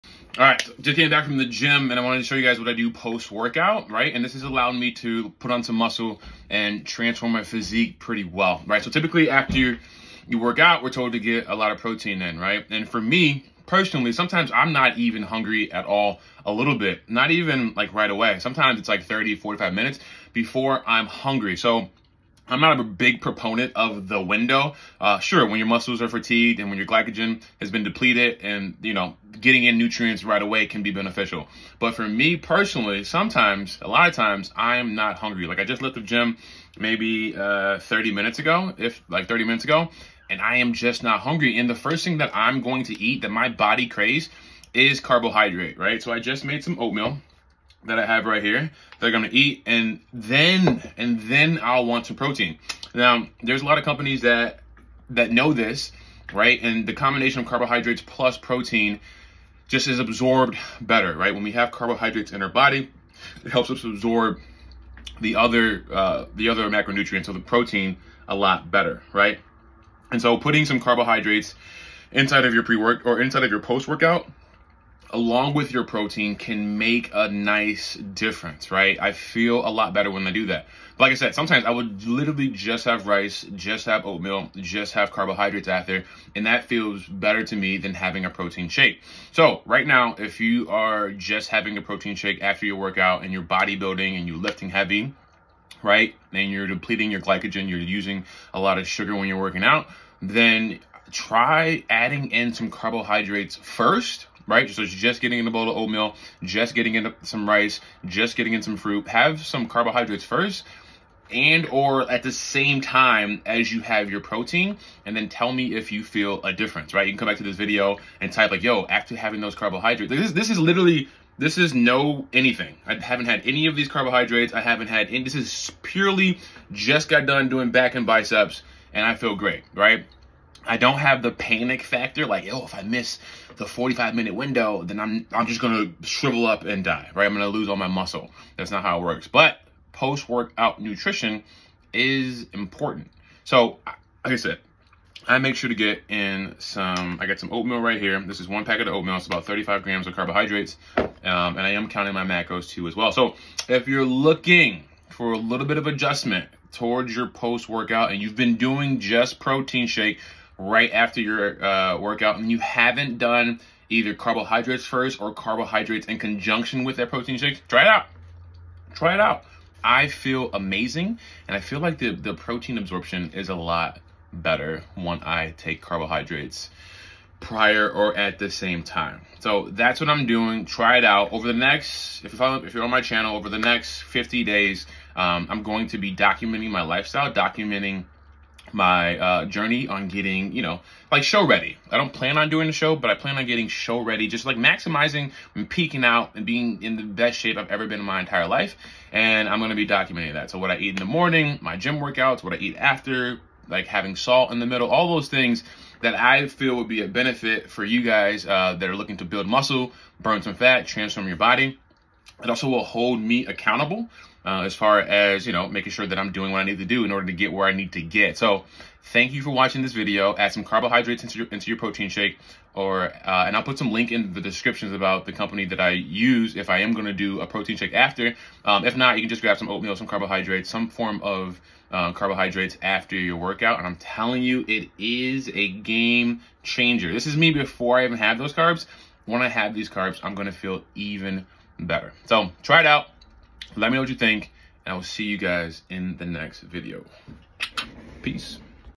Why I Stopped Doing Protein Shakes Post Workout | I Do This Instead
Protein Shakes are great and convenient but the macro split of the protein shake has made a difference.

Check out how I do my post workout nutrition and then try it. Tell me you don’t feel different/better

Also, if you want the blueprint on how I successfully train my clients then grab the blueberry below

Post Workout Stack Supplement